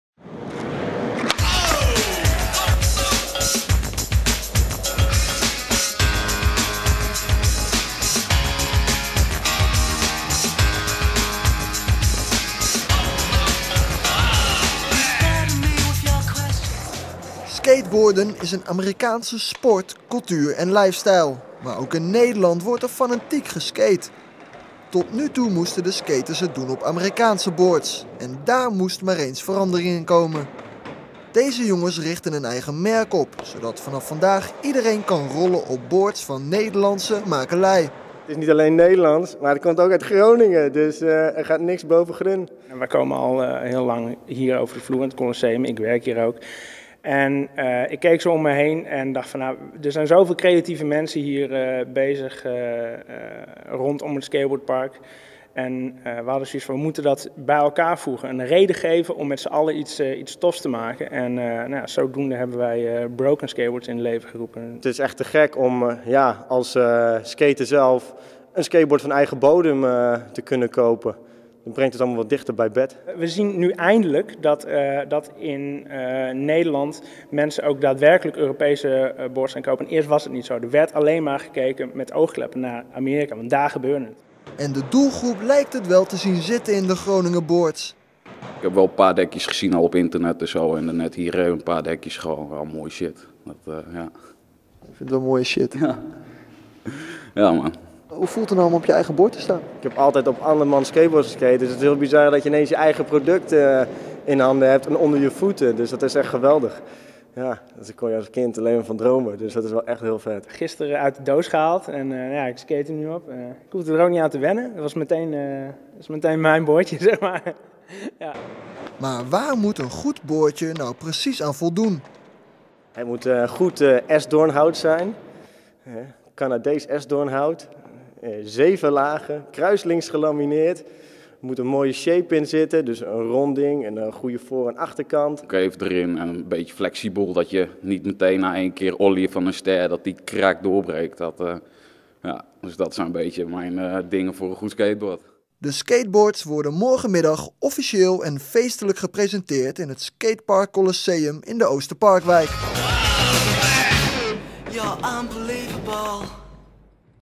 Stadjers ontwikkelen eigen skateboard.
Een aantal Groningers hebben eigen skateboardmerk ontwikkeld.